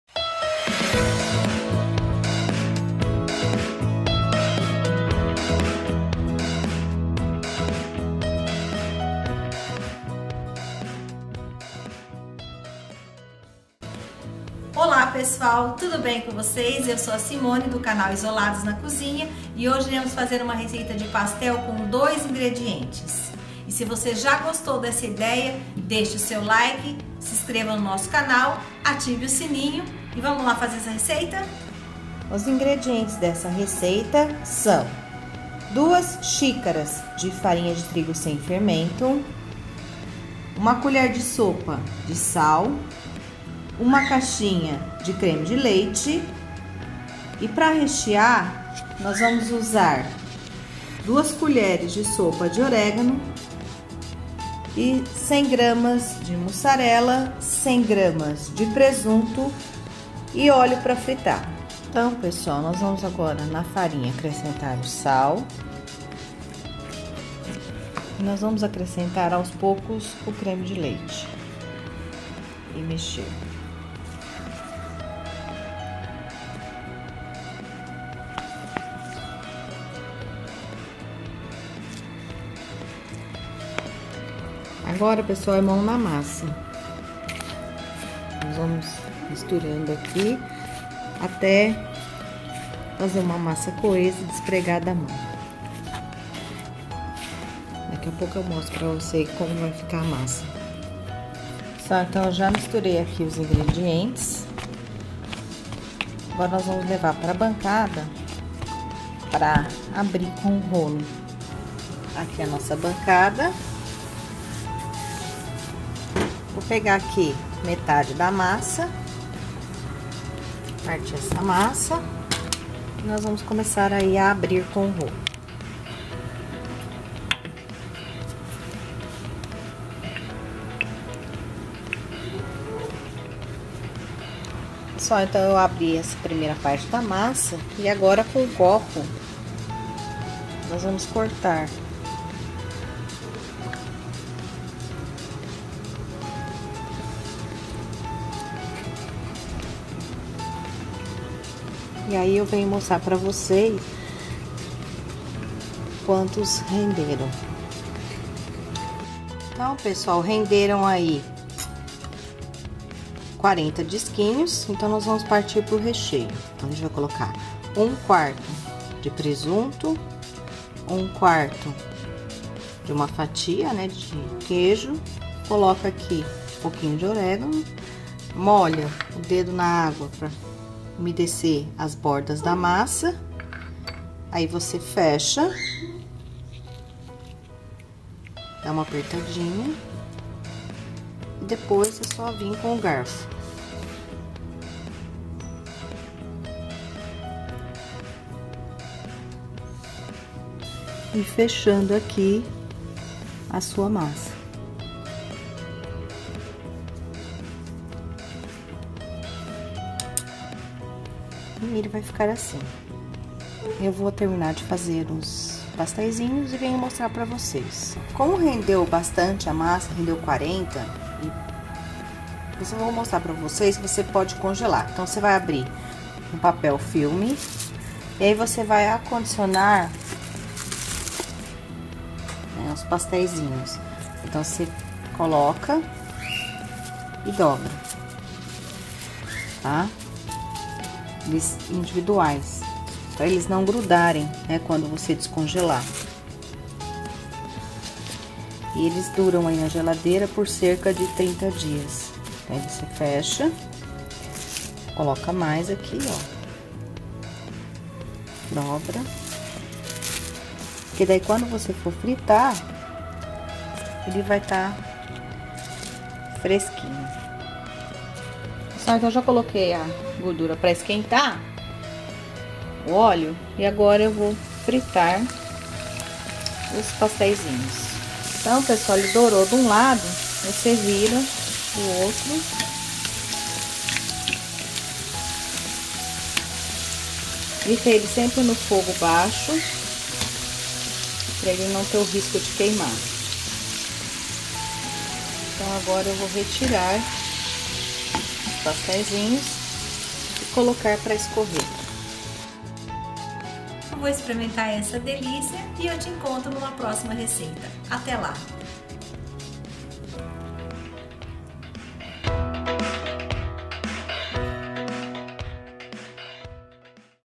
PASTEL DE CREME DE LEITE! SÓ 2 INGREDIENTES!!!
PASTEL DE CREME DE LEITE

Como fazer pastel

Ingredientes

Massa

1 caixa de creme de leite
2 xícaras de farinha de trigo

Recheio

100g de muçarela
100g de presunto